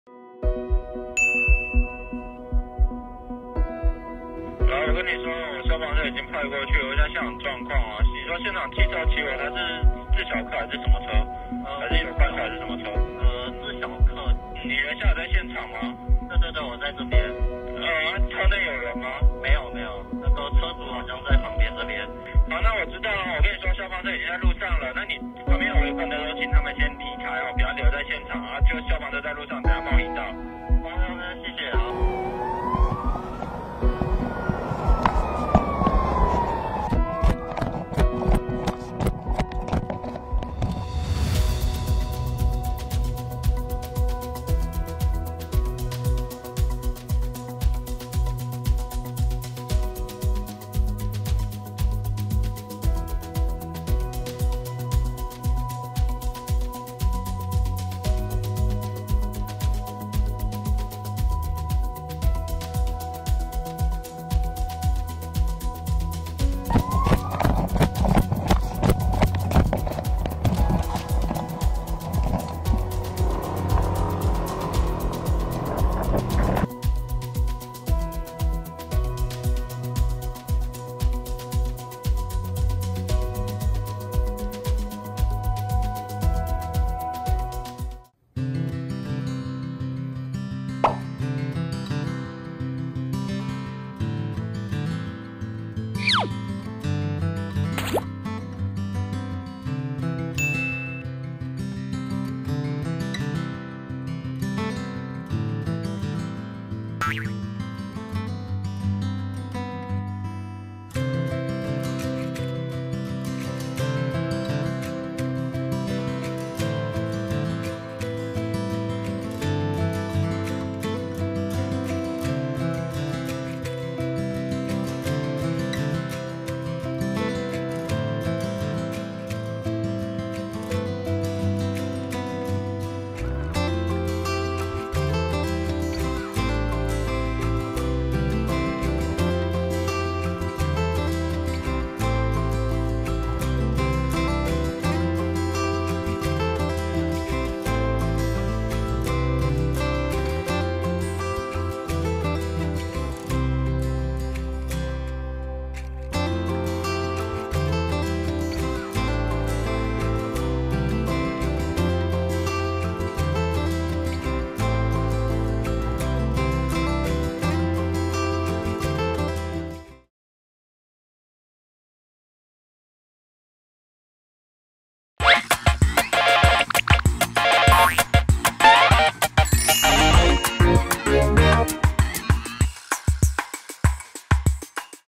2外送員送餐途中驚見火燒車 熱心報警滅火引導車輛｜新北樹林｜暖聞｜熊貓
☑影片授權：網友 莊家瑋